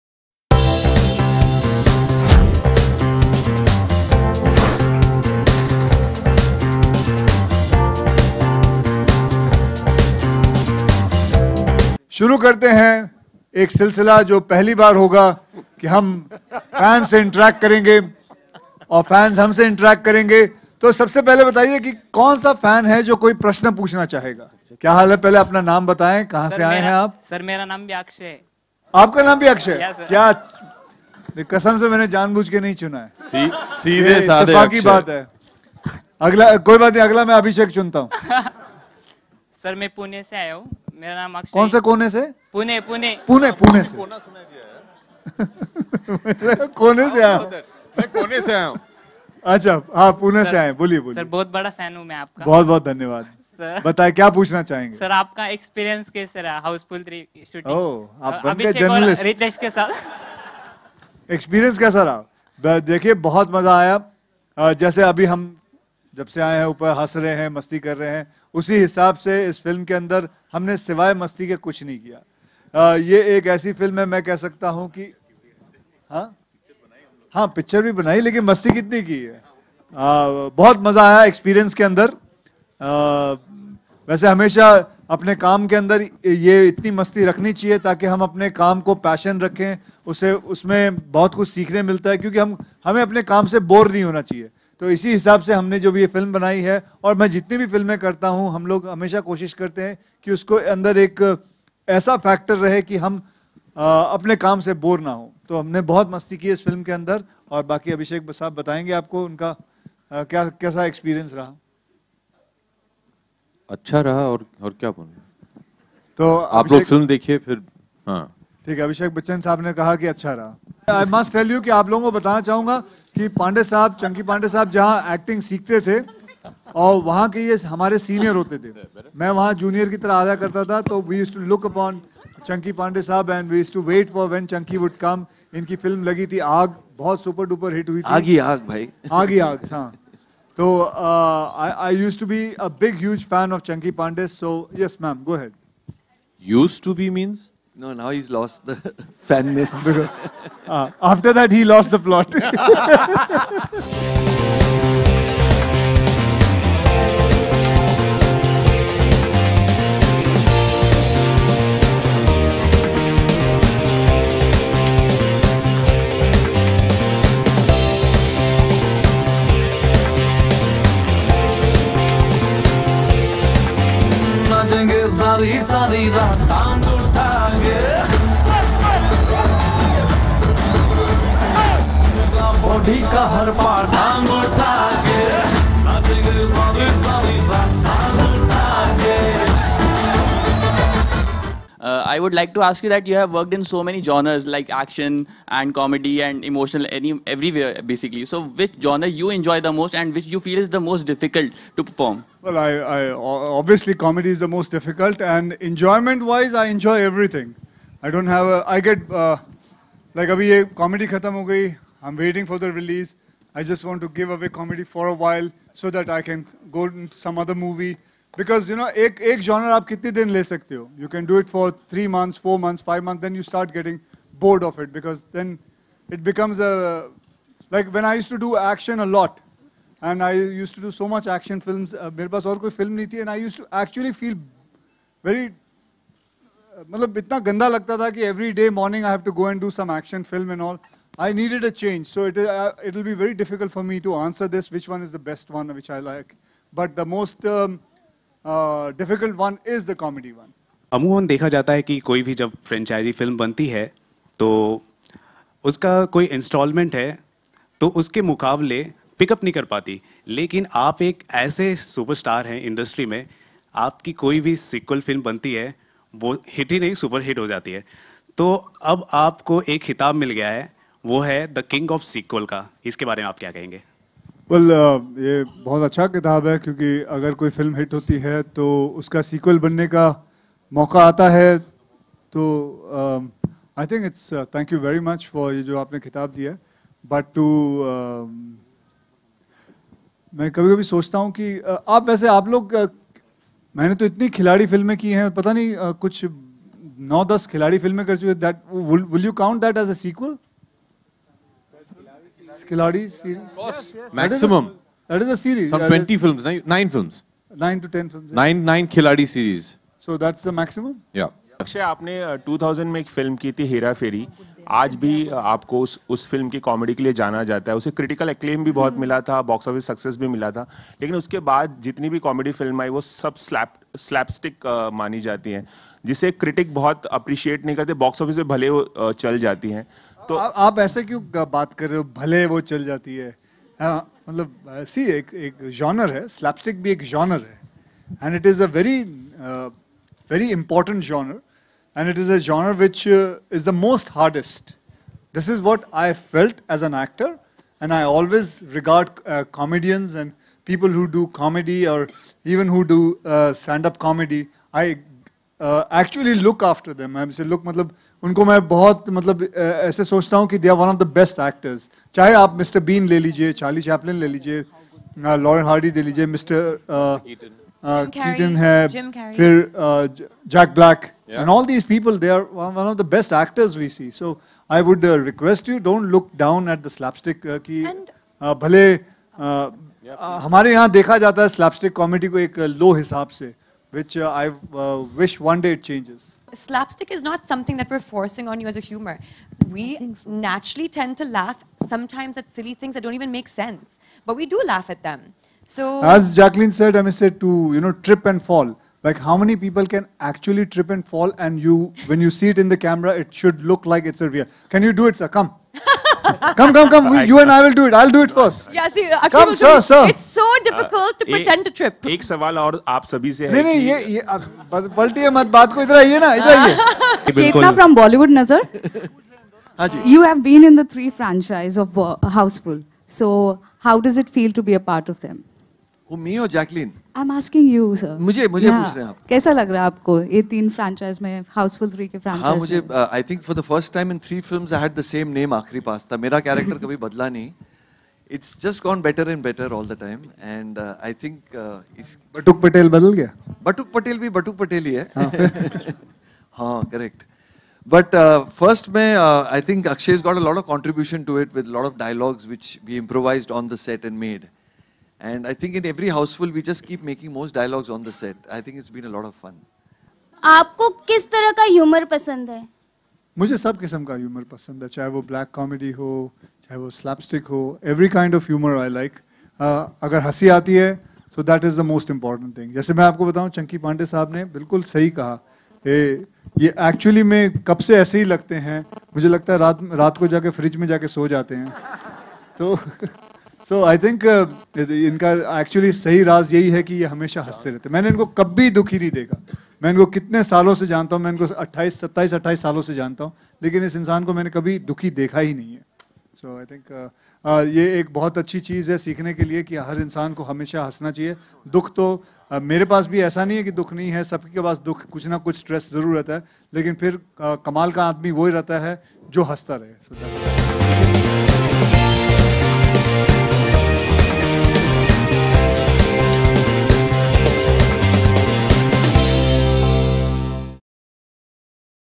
Housefull 3 team promote their movie
The team of Housefull 3 promote their movie